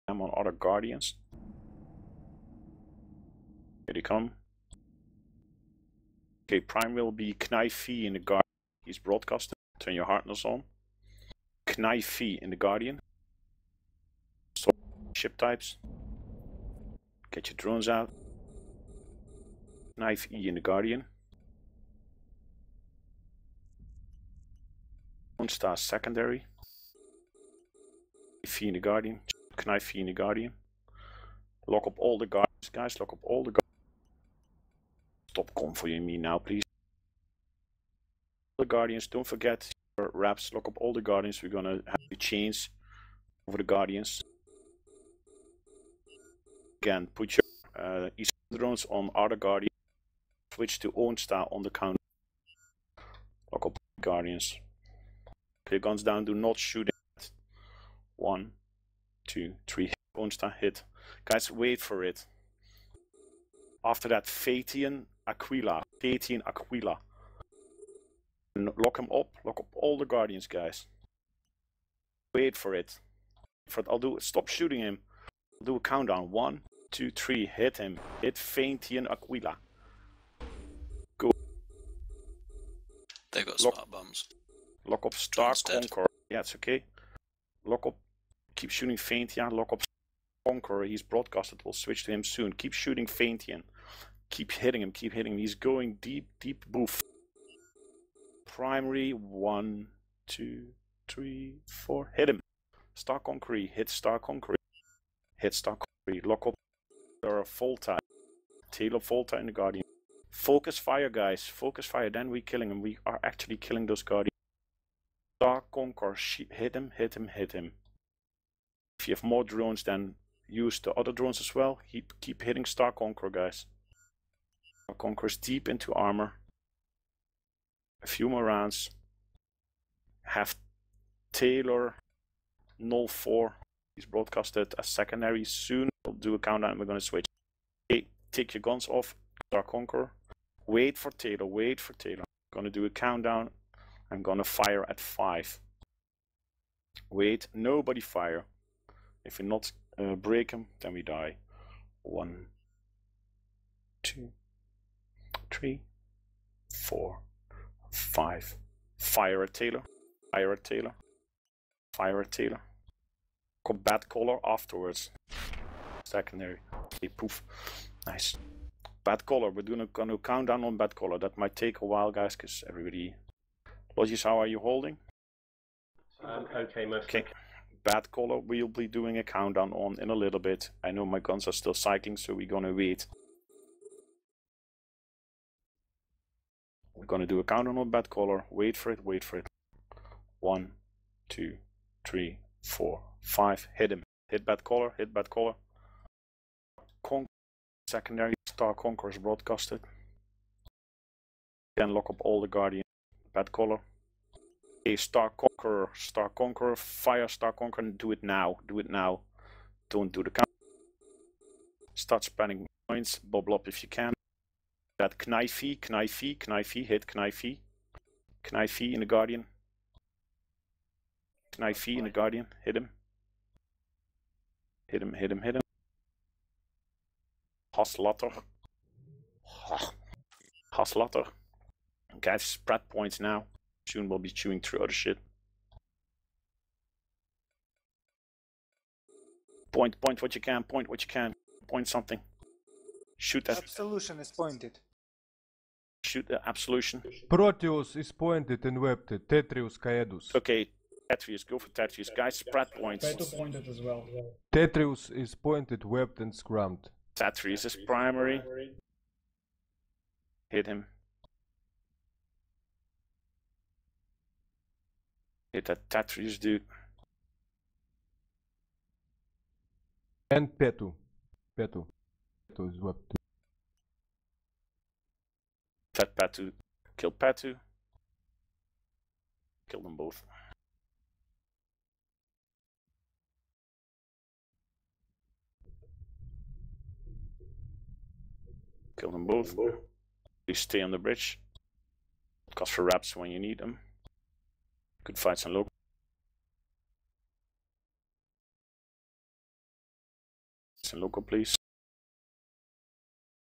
Providence vs. Verge of Collapse, F-YH5B, 2013-05-29 (unedited)
Verge of Collapse fleet pays Providence a visit in a shiny fleet. Unfortunately for them, we still had a reasonable number of people left from a previous engagement with Nulli and we hastily reship and take them on, confiscating some of their shinies